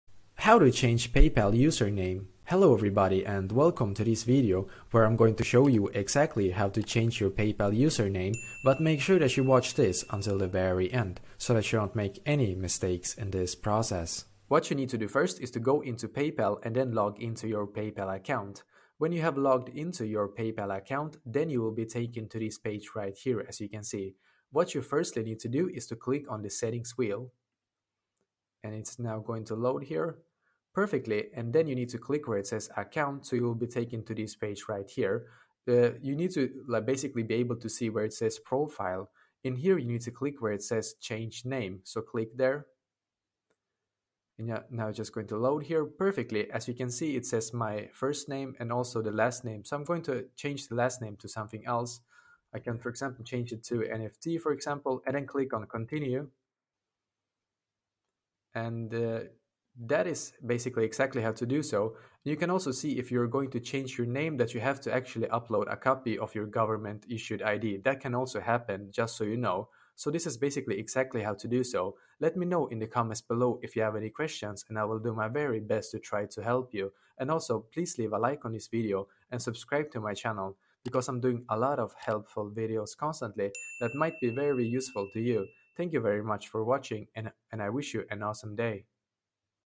How To Change PayPal Name (Change PayPal Username)
In this video I will show you how to change paypal username. After you have watched this you will know how to change your paypal name. Watch this step by step guide on how to change paypal account name.